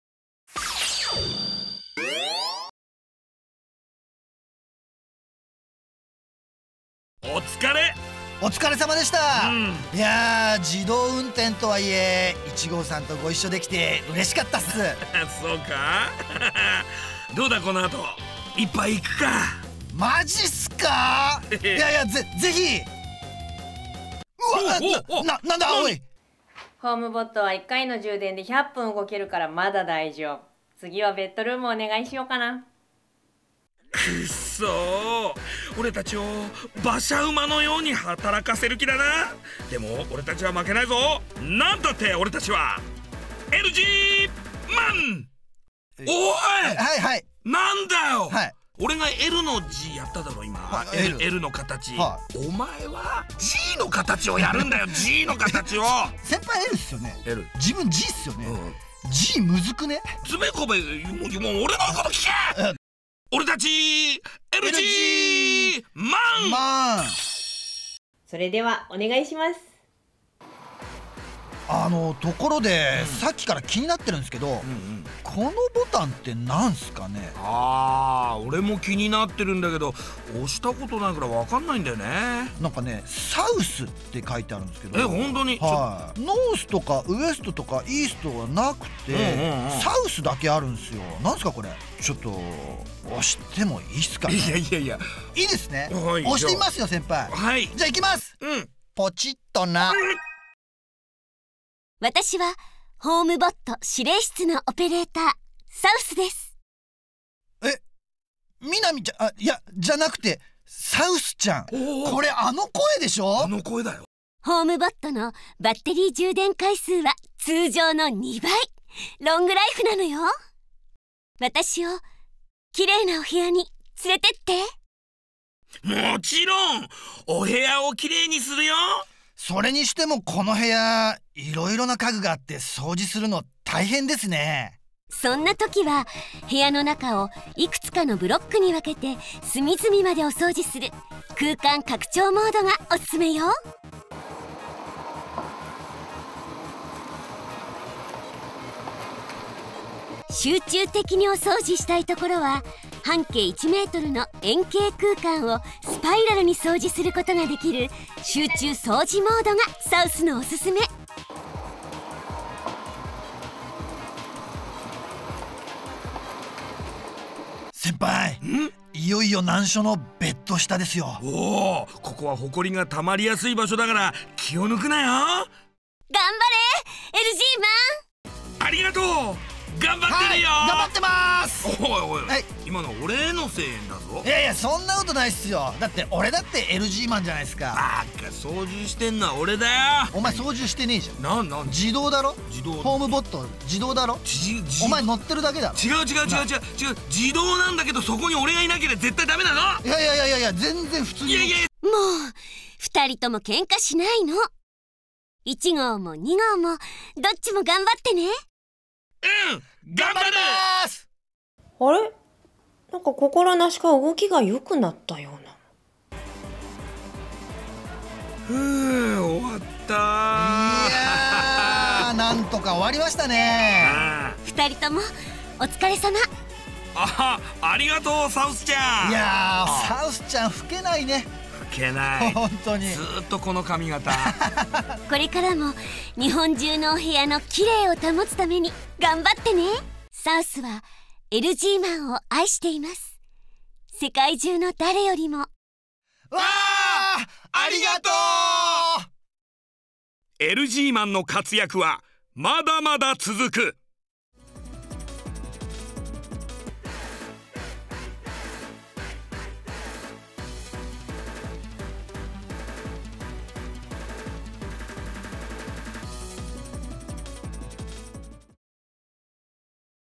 【第3話】本物の 日髙のり子 登場!その名は...ちゃん!それゆけLGマン
「本物の日高のりこ 登場！その名は・・・サウスちゃん！それゆけLGマン
　　　　　～Popular voice actor Noriko Hidaka～」

HOM-BOT2.0の発売を記念して、あの伝説のラジオ番組「鬼玉」のコンビが復活。

いよいよ、あの「タッチ」の「南ちゃん」でおなじみの国民的声優である日髙のり子さんも登場！

鬼玉のコンビと 声優 日髙のり子 さんとの強力トリオでお届けする第3話

いちばん静かで、いちばんロングライフな「自動掃除機ロボット」の機能を「わかりやすく」「おもしろく」ご覧になることができます。

「HOM-BOT（ホームボット）」は、壁際や家具、ペットにもそっと近づいて静かにお掃除します。
図書館並みに静かな掃除音48dBですから、24時間いつでもスイッチを押すだけ！

お部屋のイメージマップをつくってムダのない動きでお掃除する姿にも、きっと驚かれるでしょう。

原動力のバッテリーは、今までよりも２倍近く長持ちとなり、

さらにエコでエコノミーに進化しました。
HOM BOT2.0 商品サイト